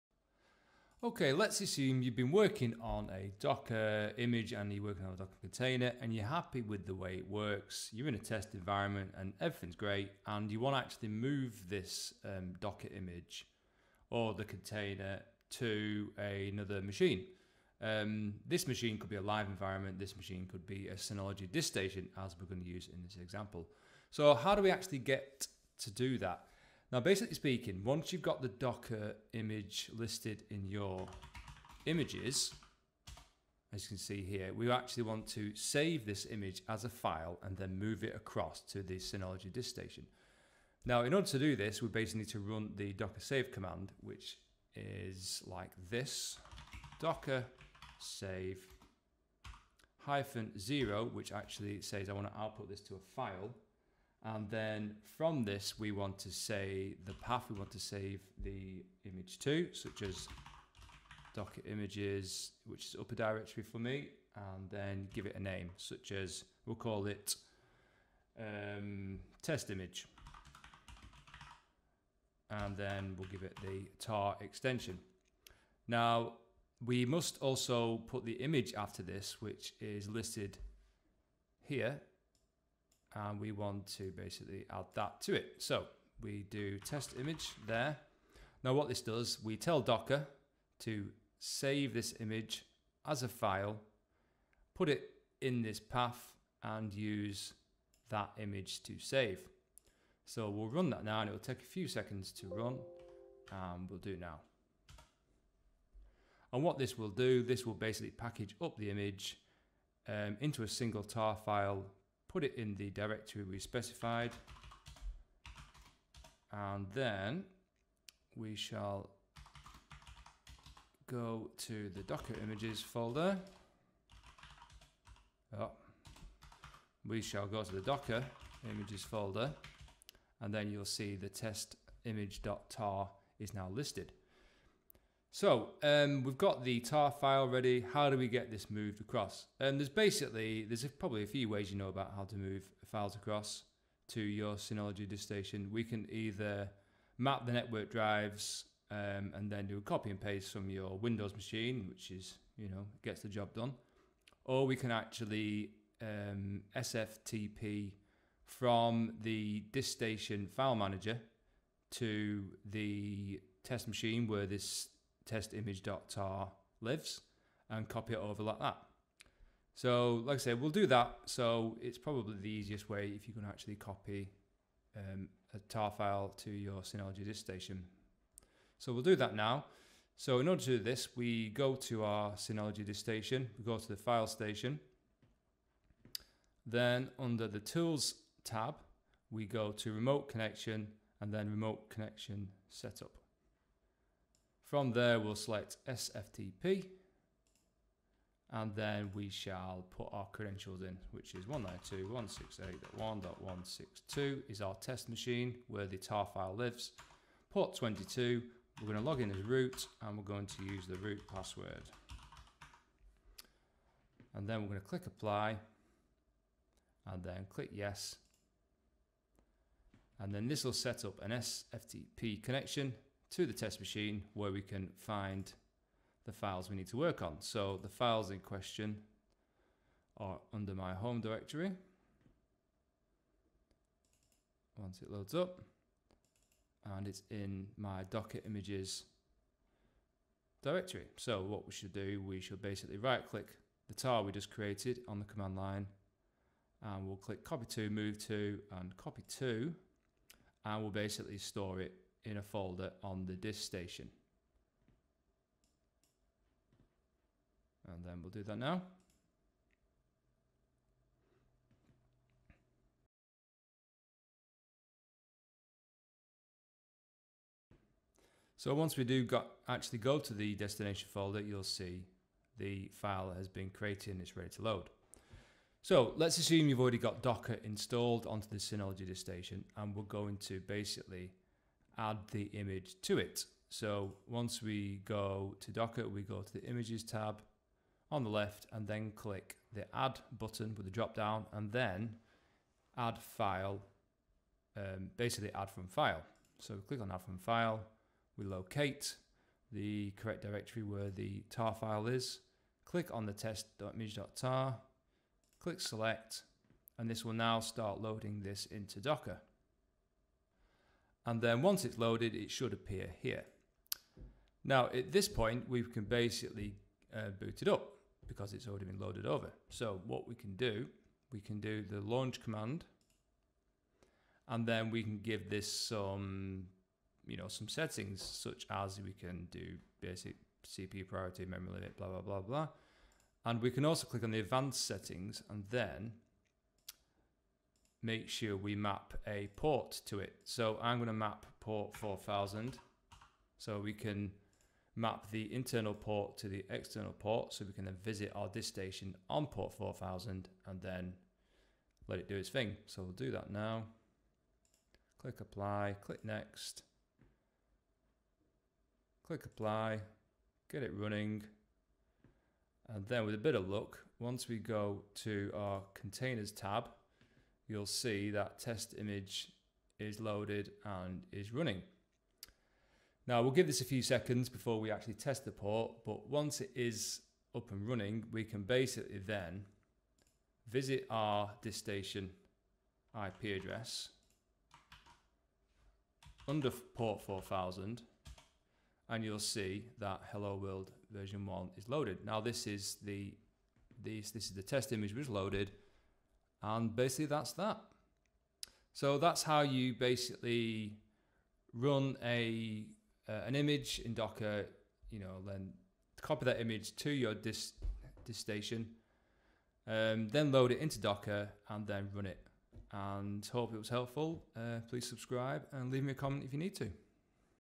Save Docker Images As File and Move to Other Machines - Synology DiskStation
A quick example of how to save Docker Images as a single file and move this to another machine ready to launch.